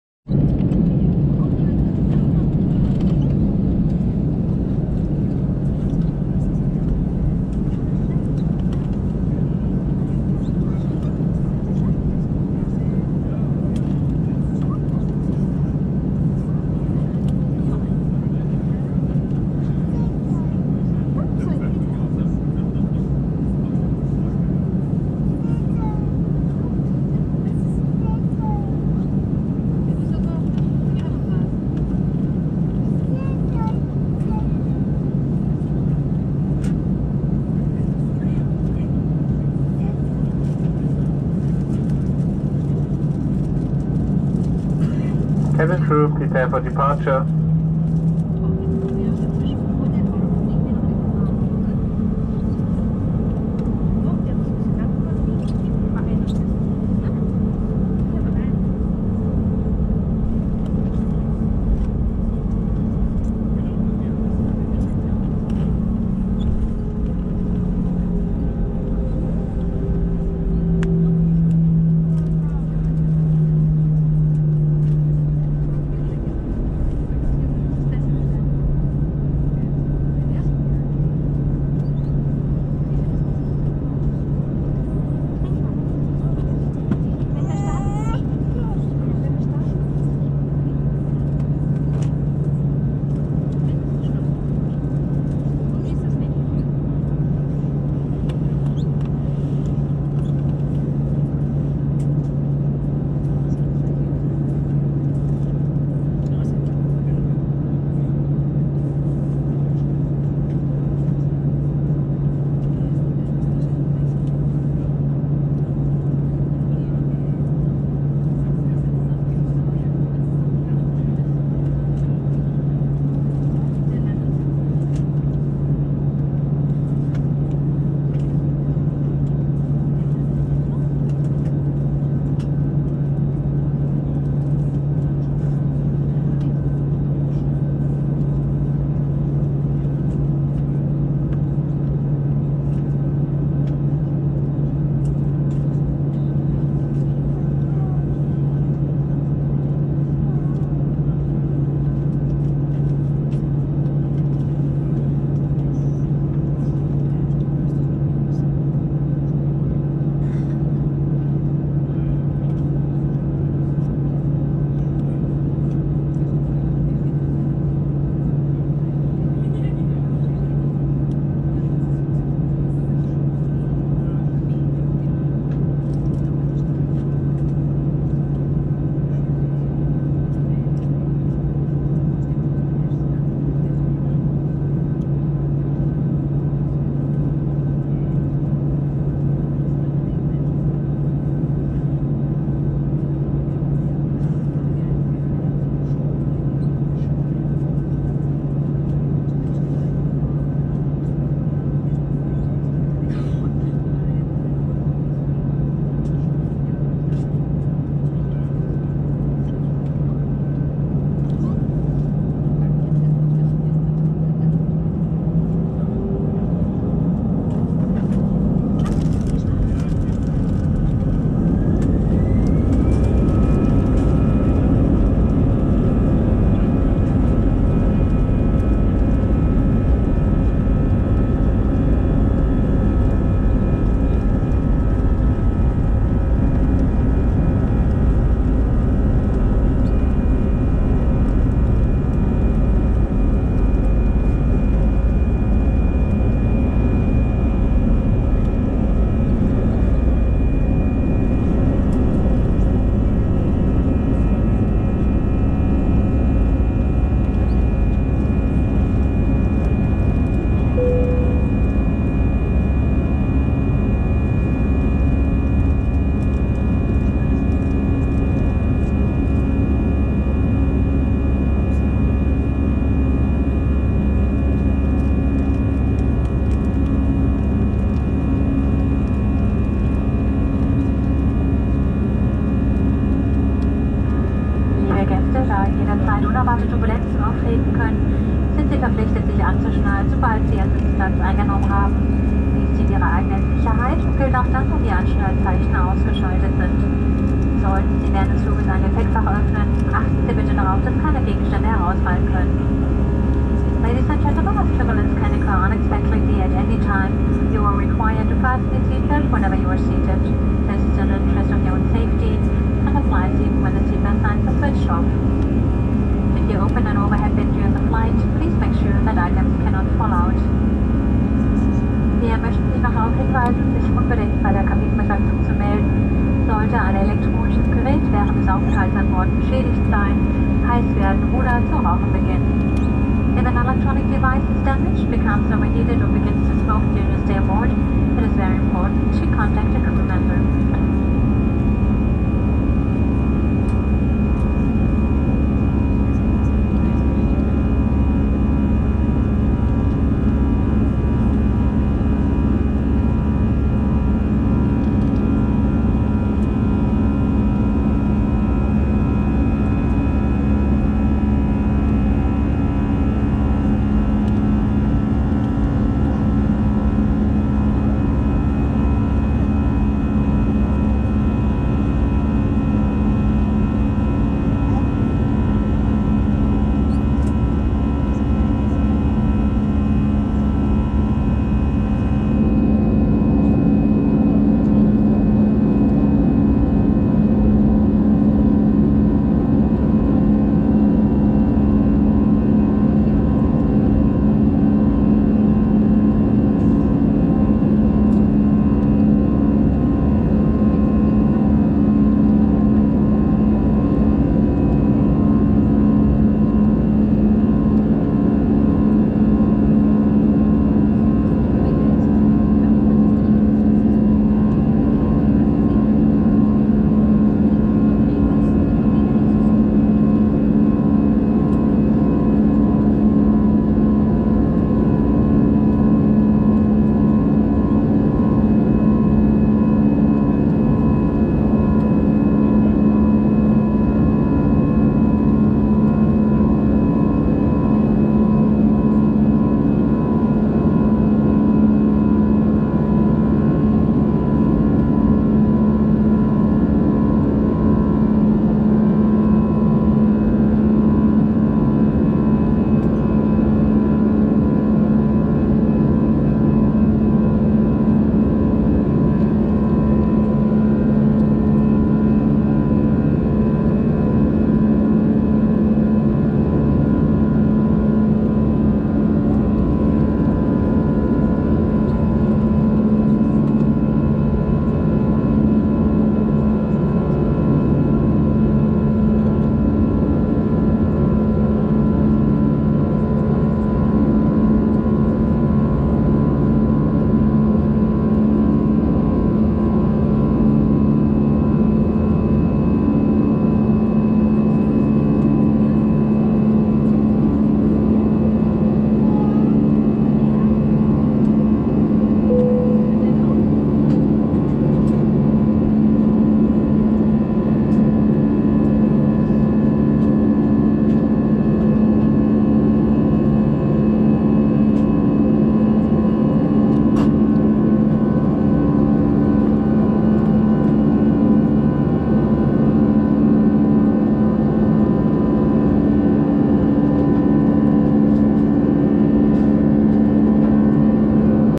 (NEW) A320 Lufthansa Takeoff at Frankfurt
Gear I am using for my videos:

GoPro 7
Suction Cup

Airport: Frankfurt/Main Airport
Airline: Lufthansa
Seat: 6A Economy
Aircraft: Airbus A320-200
Flight: LH054